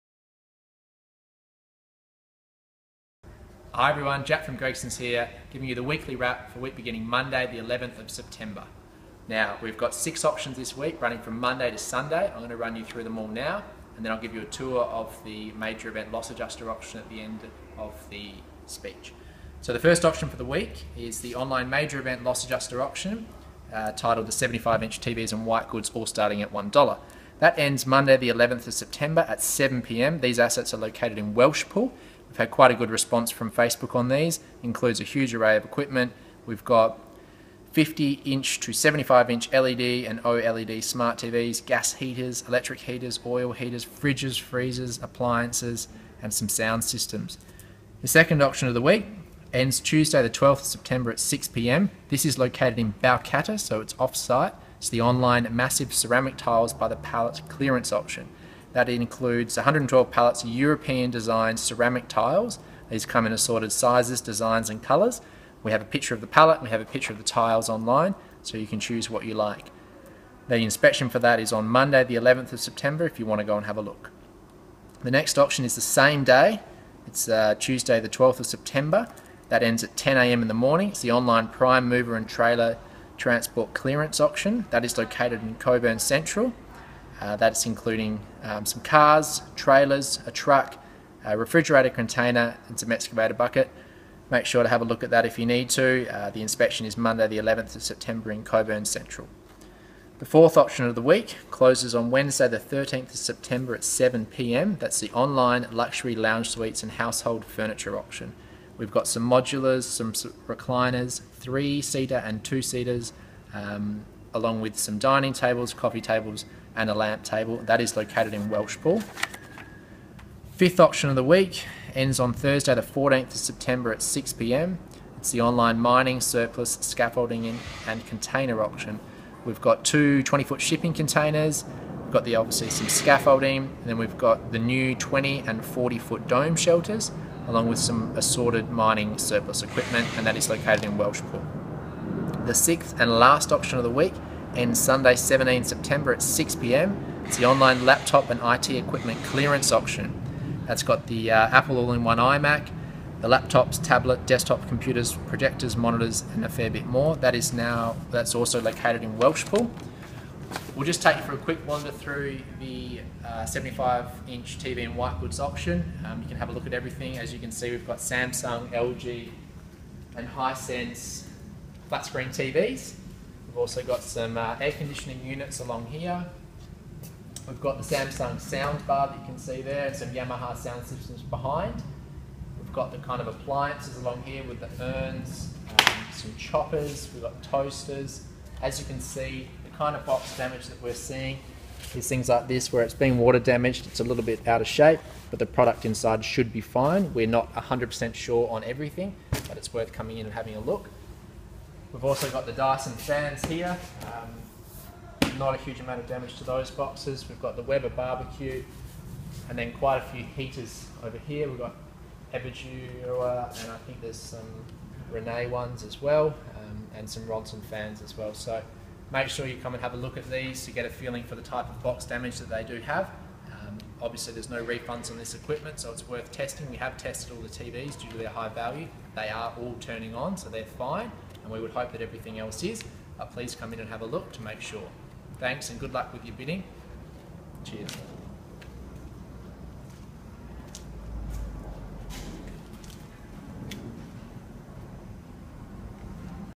The Weekly Wrap - W/B Monday 11 September 2017
Check out the latest auctions from Gregsons Auctioneers &amp; Valuers. Watch this video to get our weekly wrap on all of our items up for grabs. We have a total of 6 online auctions ending this week.

Watch the following video as we highlight this week’s products. Including 2 x Toyota Landcruisers, Western Star 4800FX 6x4 Prime Mover, Trailers, Refrigerated Container, 12 x 50" to 75" LED, OLED Smart TV's, Gas Heaters, Pallets of Ceramic Tiles in Assorted Sizes, Designs and Colours, Luxury Fabric & Leather Lounge Settings, Laptops, Desktops, Scaffolding, EW 2 x 40ft & 2 x 20ft Dome Shelters, Power Boards, Portable Switch Boards and much more.

- The first Online Major Event Loss Adjuster Auction - 75 inch TV's & Whitegoods All Starting at $1 ends on Monday, 11 September 2017 at 7pm,
- Auction 2 Online MASSIVE Ceramic Tiles by the Pallet Clearance Auction - Balcatta ends on Tuesday, 12 September 2017 at 6pm,
- Auction 3 Online Prime Mover & Trailer Transport Clearance Auction - Cockburn Central ends on Tuesday, 12 September 2017 at 10am,
- Auction 4 Online Luxury Lounge Suites & Household Furniture Auction ends on Wednesday, 13 September 2017 at 7pm,
- Auction 5 Online Mining Surplus, Scaffolding and Container Auction ends on Thursday, 14 September 2017 at 6pm and
- Auction 6 Online Laptop & IT Equipment Clearance Auction ends on Sunday, 17 September 2017 at 6pm

Visit our warehouse in Welshpool to check out the items or view the items on our website today. All bidding can be done through our online bidding platform by going to our website and clicking on the ‘Buy’ section. Don’t miss this great opportunity to grab a deal. Be sure to register to start bidding.